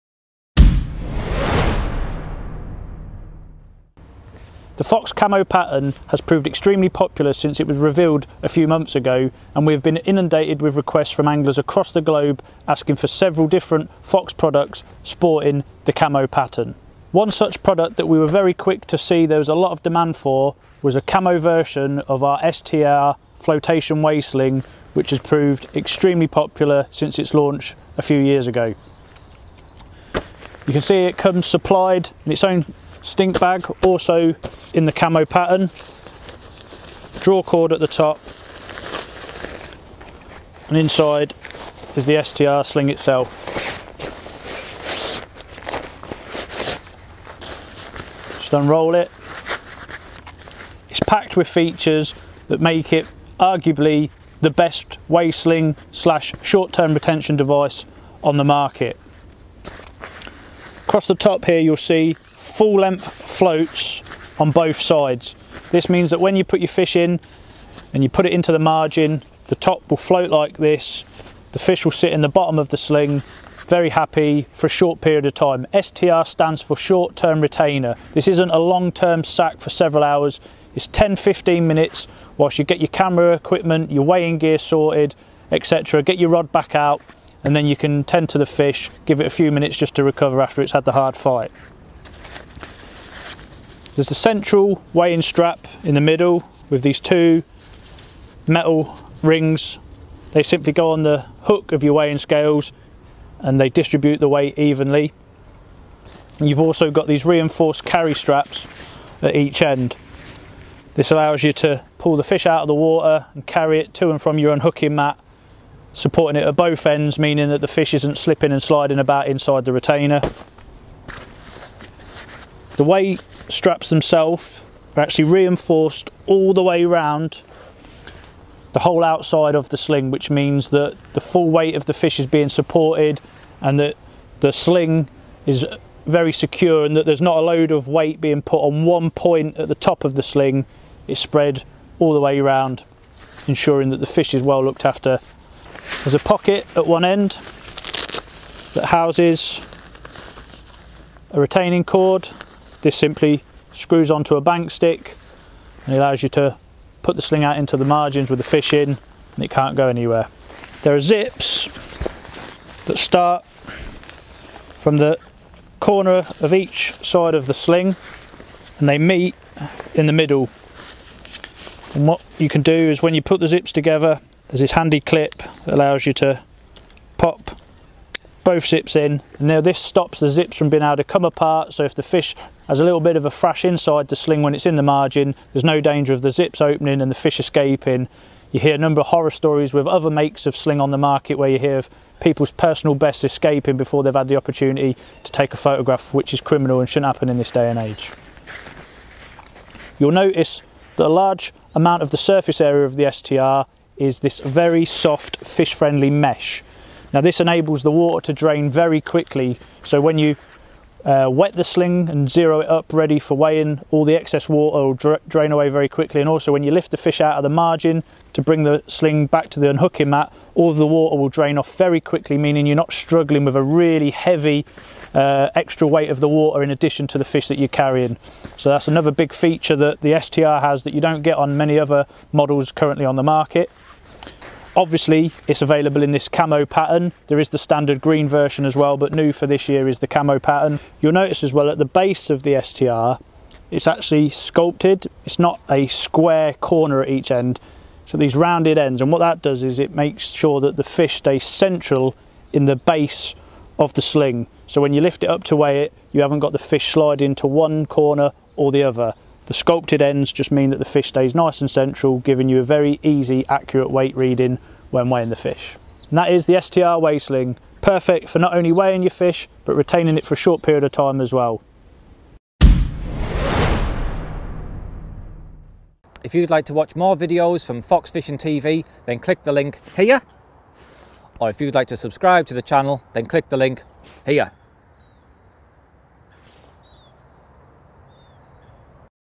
***CARP FISHING TV*** NEW Camo STR Weigh Sling
Our market-leading STR Floatation Weigh Sling has now been launched in our unique camo pattern and is packed with features...